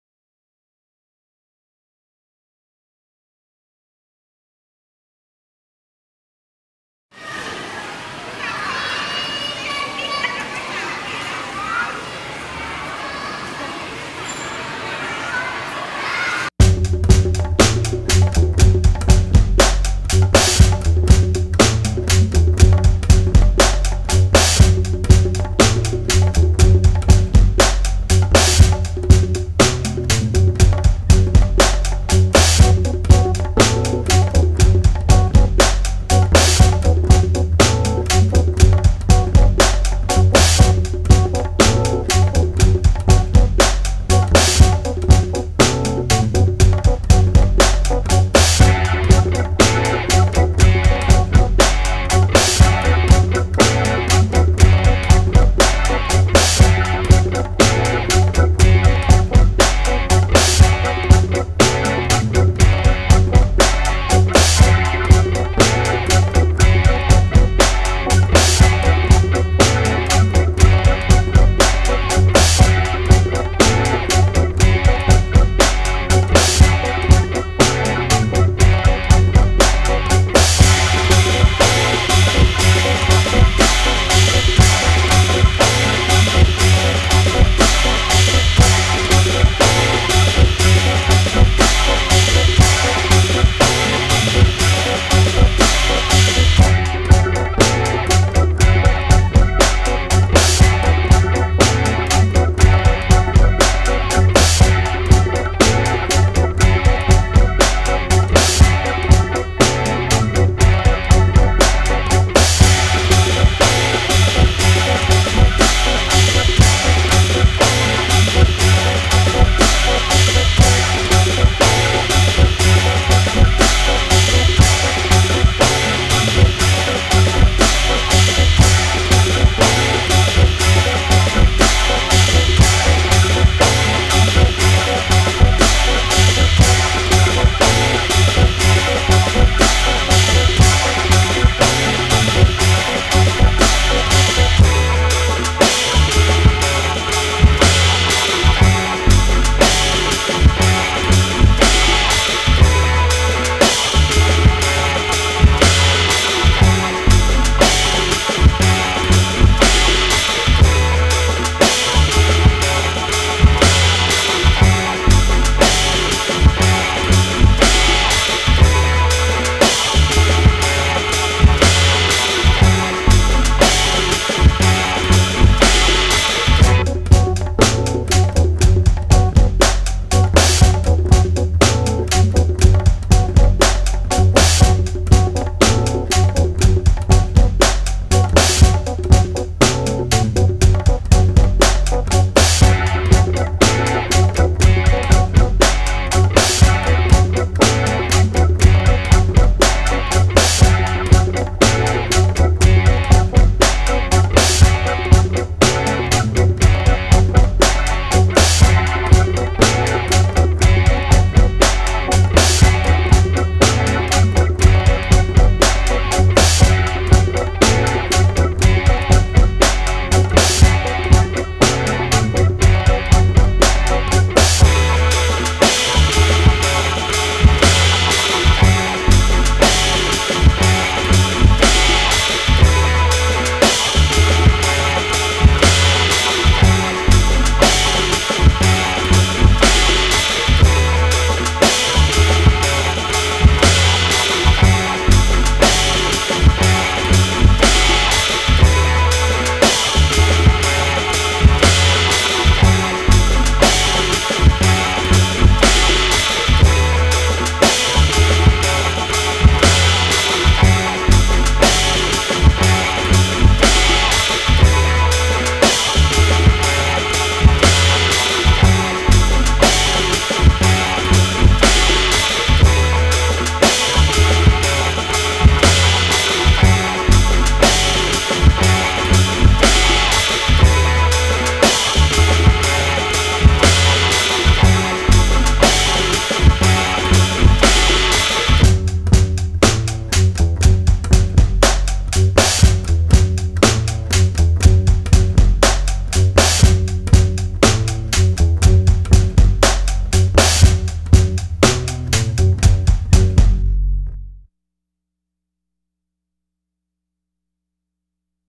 Basketball Creative Dance by Kinder 2 Glory 2 2pm Gil Camporazo
MLKS Kinder 2, Glory 2 (class 1pm) creative dance contest entry was named the Champion during the 43rd Founding Anniversary of Mama's :Love Kinder School, La Carlota City, 15 February 2018.